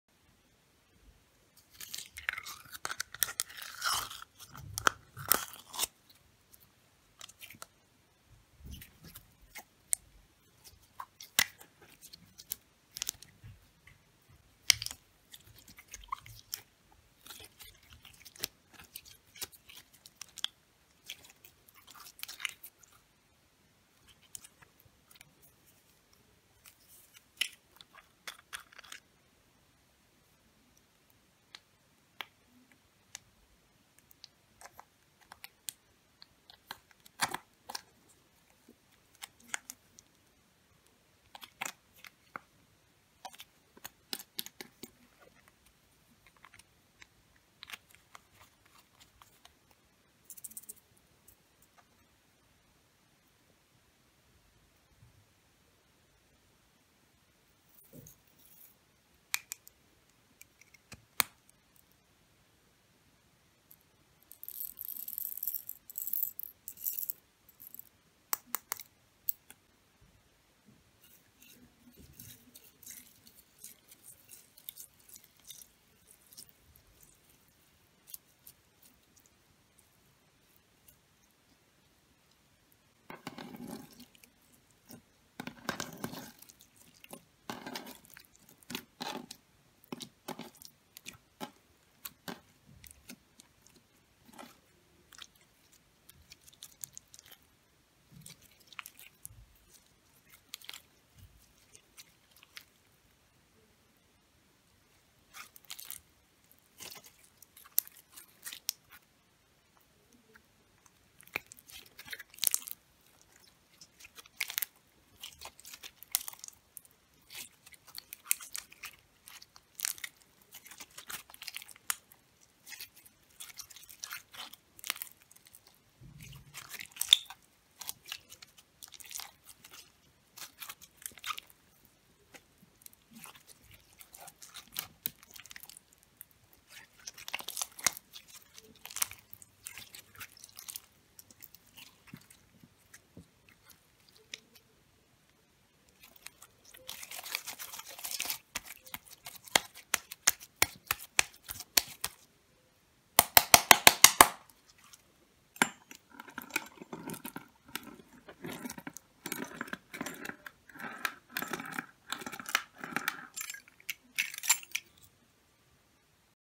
Mixing clear slime with colorful glitter and liquid lip gloss Satisfying ASMR video/Natalie Slime
Hi guys))
I mixed transparent glass slime with multi-colored glitter and liquid blue lip gloss))